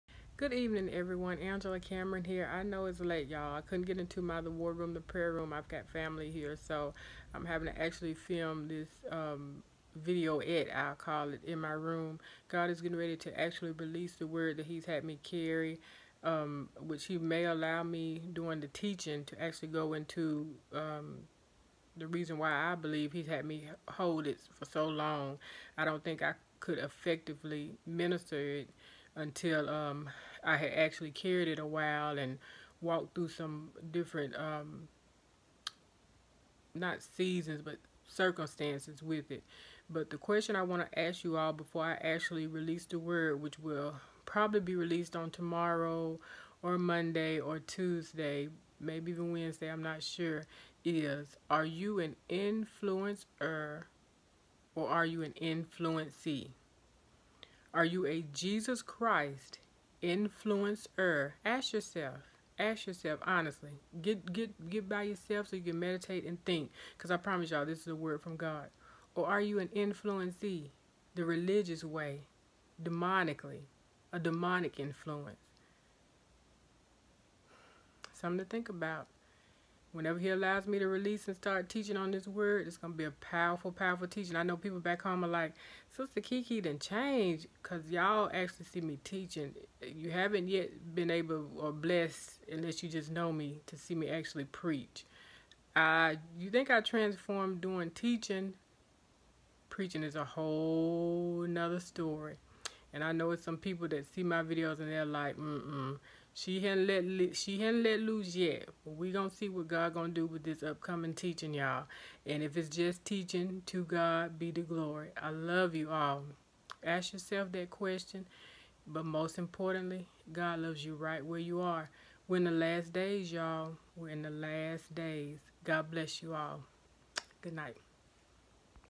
ARE YOU AN INFLUENCER OR INFLUENCEE (demonic/religious/“world” way of doing things MINDSETS (MY GOD)
ARE YOU AN INFLUENCER OR INFLUENCEE (demonic?/religious?/“world” way of doing things?) MINDSETS (miiiiiinnndsets) IS HOW I HEAR IT. MY GOD🙇🏽‍♀️MY GOD, MY GOD🔥✨🕊🙌🏽

“I PERSONALLY THANK GOD🙌🏽ALMIGHTY FOR ALL OF THE PEOPLE HE PLACED IN MY OWN LIFE TO HELP LEAD AND USHER ME TO MY DIVINE PLACE OF PURPOSE. MY DIVINE PLACE OF DESTINY; THAT THEY DID NOT LOOK THROUGH THE “EYES” 👀of RELIGIOUS VIEWS AND THAT GOD “REVEALED✨” AND “UNVEILED✨” HIS HAND UPON MY LIFE. I AM SO DEDICATED TO HELPING OTHERS “LIKE ME” REACH THEIR GOD GIVEN GOALS. MINISTRY HAS ALREADY BEGAN ON “THE STREET” I THANK GOD FOR CHOOSING, USING, AND PREPARING ME FOR SUCH A TIME AS THIS. I AM NOT WORTHY BUT STILL I AM VERY, VERY, VERY GRATEFUL THAT THE BLOOD OF JESUS HAS WASHED ME WHITE AS SNOW MAKING ME WORTHY THROUGH OUR LORD AND SAVIOR JESUS CHRIST.” “YOUR GLORY FATHER GOD. YOUR GLORY.”

IT IS GOD’ S JOB TO JUDGE. THE HOLY SPIRIT’ S JOB TO CONVICT. MY JOB TO LOVE ❤️.

ALL THE REST OF YA’ LL CAN DO IT YOUR OWN WAY JUST ALWAYS REMEMBER WE ALL HAVE SINNED AND FALLEN SHORT OF THE GLORY OF GOD-ROMANS CHAPTER 3:23. REMEMBER THIS THE NEXT TIME SOME OF YA’ LL WANT TO STEP OUTSIDE OF LOVE AND CONDEMN..NOT REBUKE...CONDEMN OR POINT THE FINGER AT A “BABE IN CHRIST” WHAT “STATE” DID HE FIND YOU IN OR DELIVER YOU FROM? OR STILL DELIVERING YOU FROM???
COME ON...BE HONEST. 💣✨

👠divafaithproductions🥰✝️💋